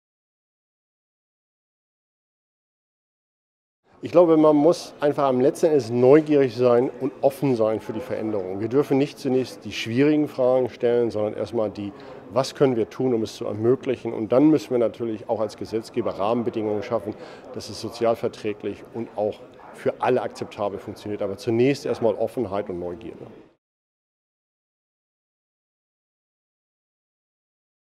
Post-CEO Appel: Wir müssen der Digitalisierung mit Neugier begegnen
Beim Denkraum Digitale Soziale Marktwirtschaft am 11. Juli 2018 in Bonn diskutierten rund 200 Vertreterinnen und Vertreter aus Politik, Wirtschaft und Zivilgesellschaft über die Digitalisierung und die Auswirkungen auf unser Wirtschaftsmodell. Zu den Fragen, wie sich unsere Arbeitswelt durch die Digitalisierung verändert und welche Rolle der Staat dabei hat, gab es unter den Podiumsteilnehmerinnen und -teilnehmern unterschiedliche Meinungen. Dr. Frank Appel, CEO der Deutschen Post DHL Group, appellierte an die Neugier der Menschen gegenüber der Digitalisierung und den daraus folgenden Veränderungen.

Der Denkraum Digitale Soziale Marktwirtschaft mit dem Hauptpartner Deutsche Post DHL Group war die achte Denkraum-Veranstaltung. Der Denkraum bringt Entscheiderinnen und Entscheider aus Politik und Wirtschaft, zivilgesellschaftliche Multiplikatorinnen und Multiplikatoren und interessierte Bürgerinnen und Bürger zusammen. Initiatoren des Denkraum sind die Frankfurt Business Media GmbH und die IFOK GmbH.